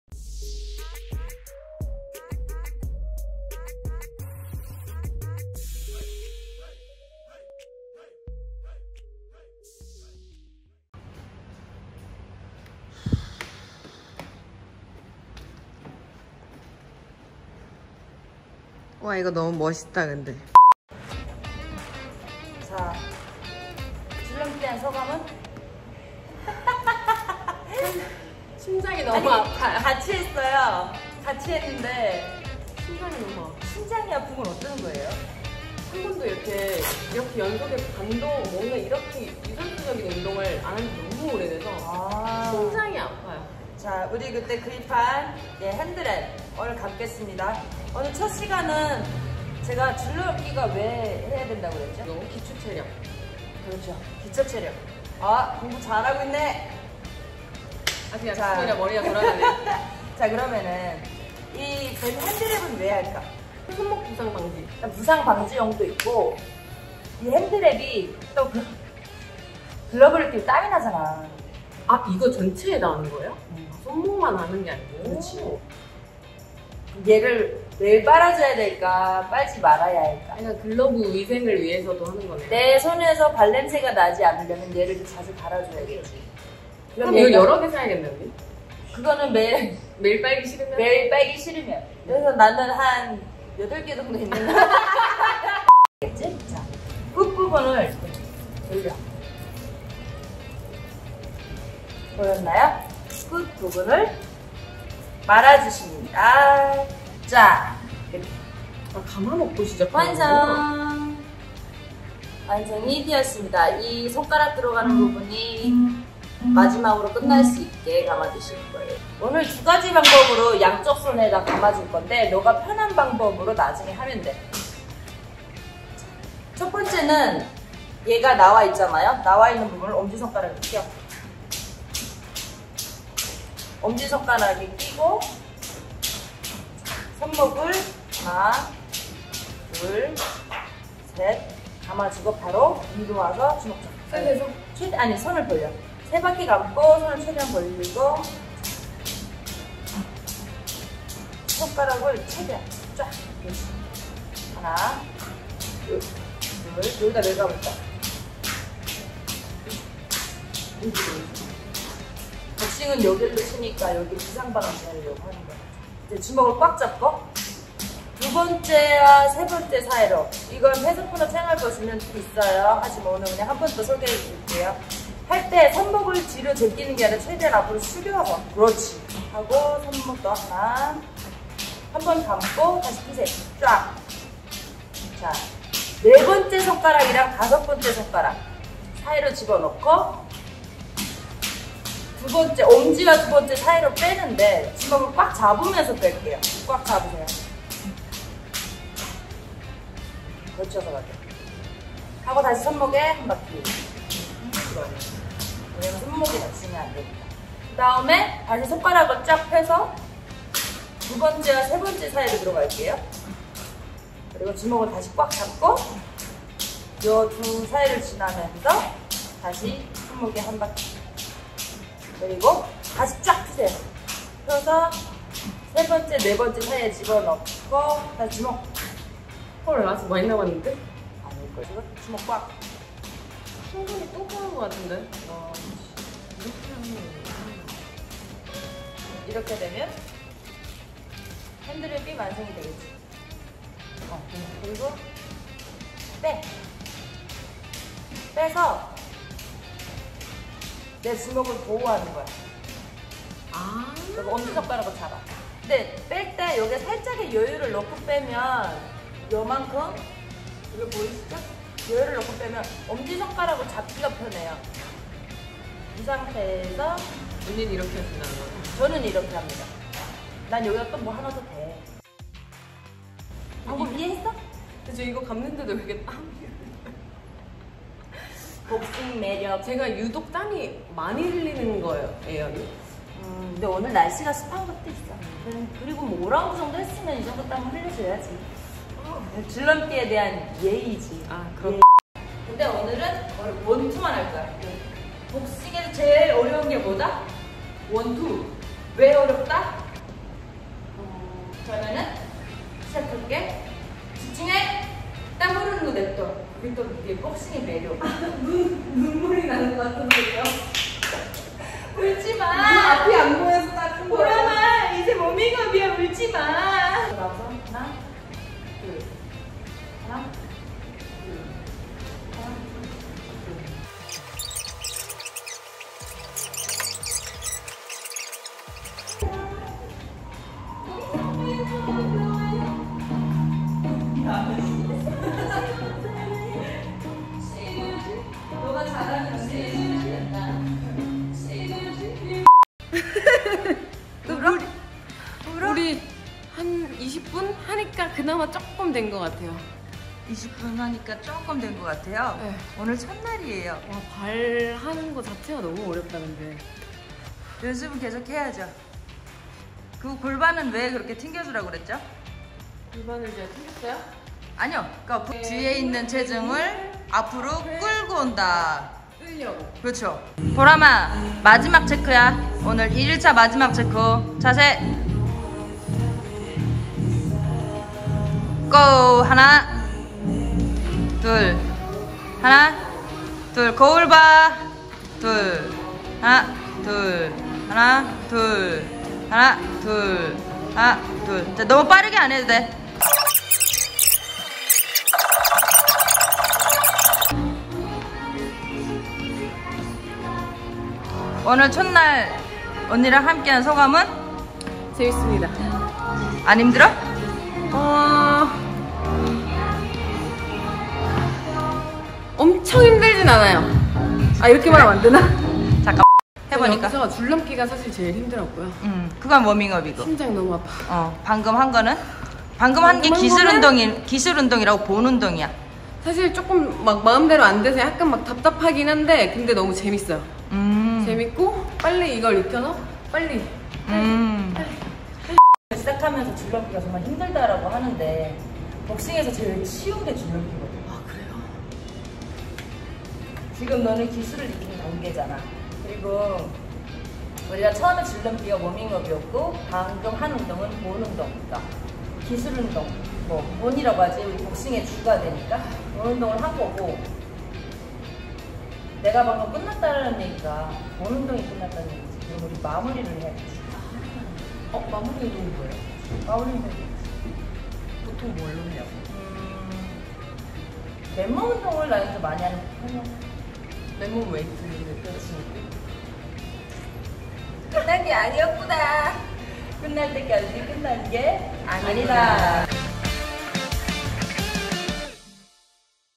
WBA챔피언 최현미의 첫 제자! 돼람이 복싱 1일차!!
안녕하세요 패서프로 최현미입니다.
돼람이의 복싱1일차.
궁금하시죠?^^

많은 시청 부탁드리고 많은 응원부탁드립니다
감사합니다^^

인스타그램 ID : @hyunmi.choi.7